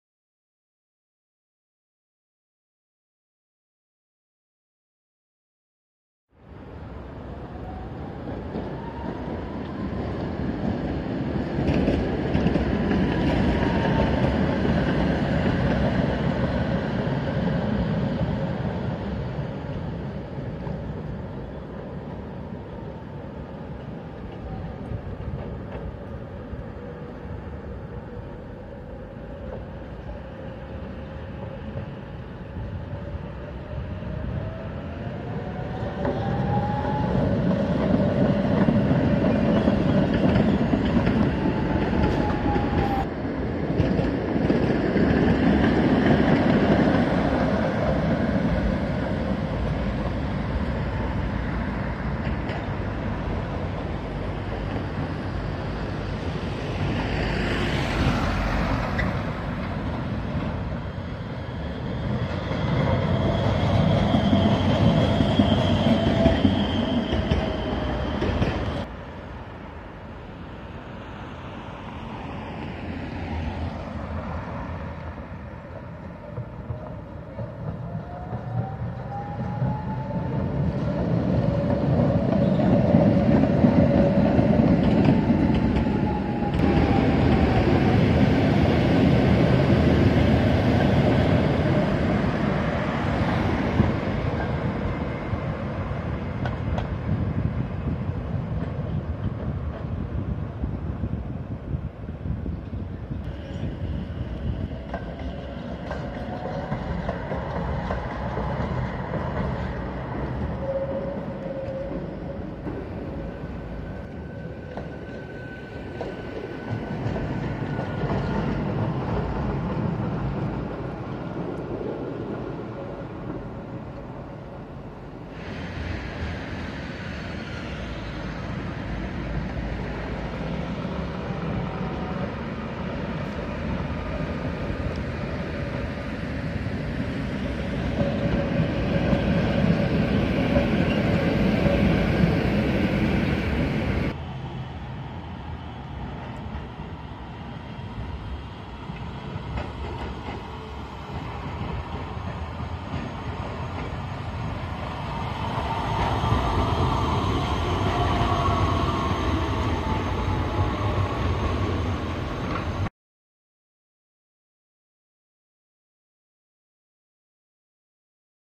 BRNO_PROVOZ NA ZÁBRDOVICKÝM MOSTĚ
V pátek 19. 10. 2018 jsem se vypravil kouknout na Zábrdovický most v Brně Židenicích. Tento den je posledním dnem před výlukou, kdy přes most ještě jezdí auta. Od soboty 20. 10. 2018 už tomu tak nebude a od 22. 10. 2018 neprojedou ani tramvaje (čtěme šaliny). Důvodem je nejen oprava Zábrdovického mostu.
Konec oprav zhruba za 15 měsíců.